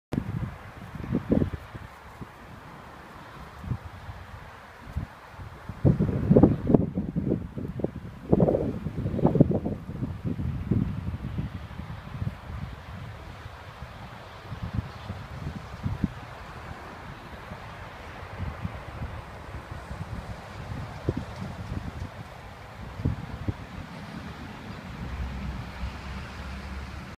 Spring in Mittagong
We visited to our uncle's house in Mittagong during early spring in 2013. This was taken under a Cherry Blossom tree in front of St Stephen's Anglican Church, where the flowers from the tree were dropping down. Mittagong is a small, lovely town in Southern Highlands, NSW, Australia.